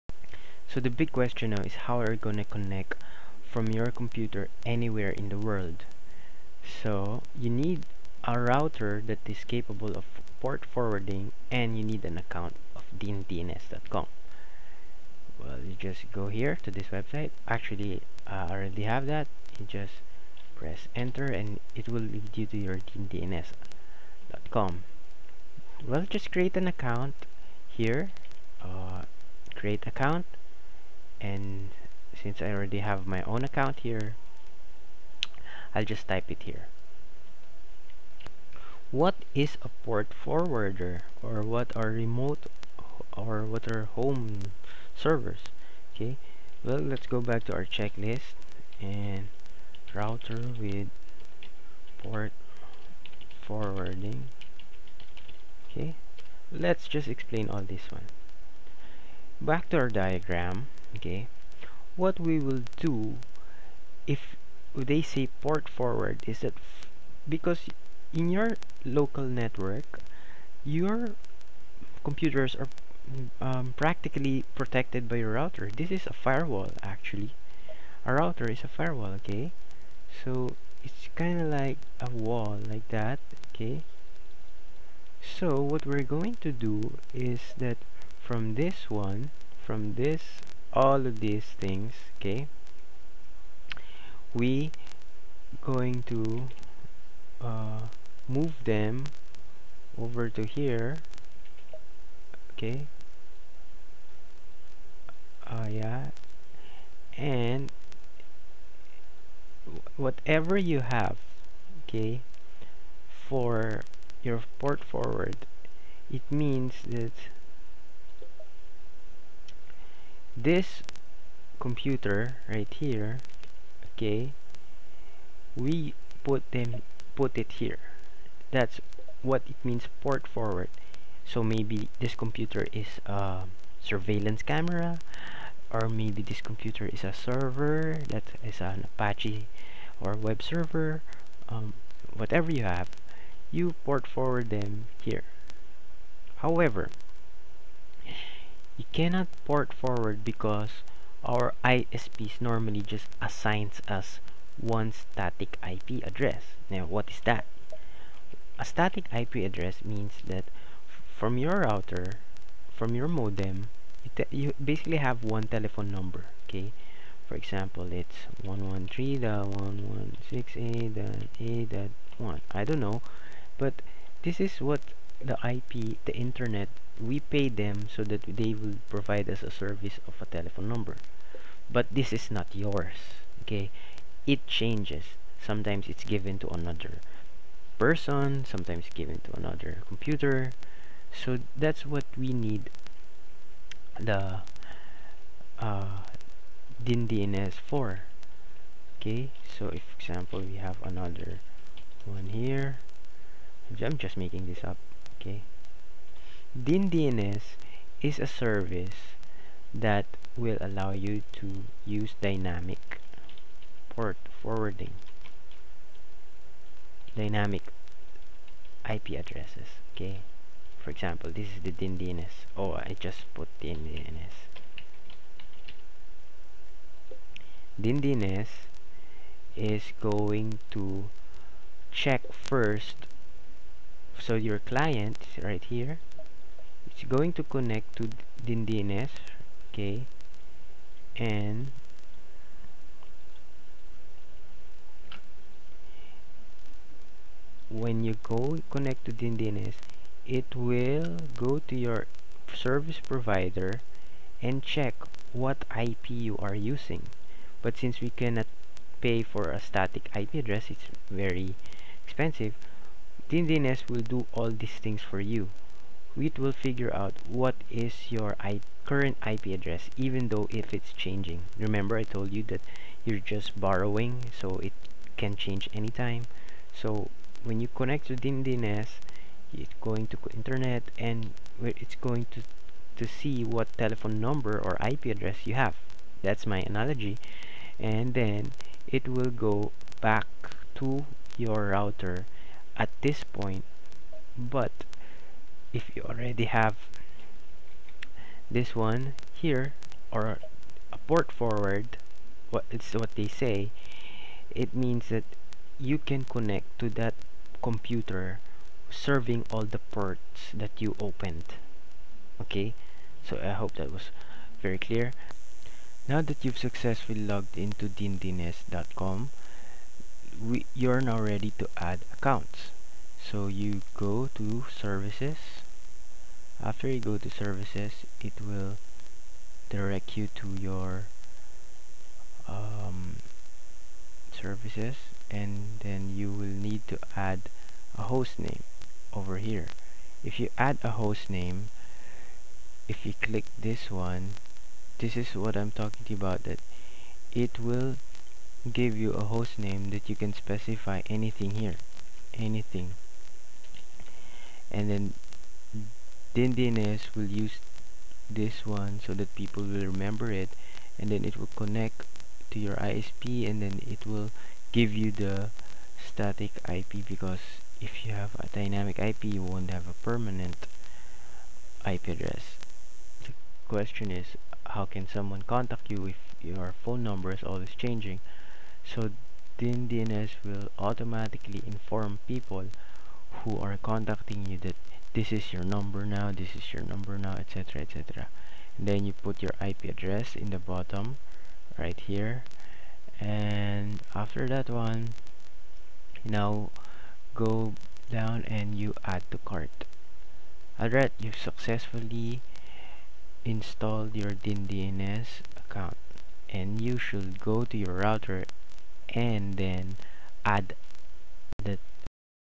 Connect Remote Computer - SSH & FreeNX Part 4/5
What is port forwarding? We'll explain it on this video and setup a DYNDNS.COM account so people can access your LAN even if you're in a dynamic IP environment.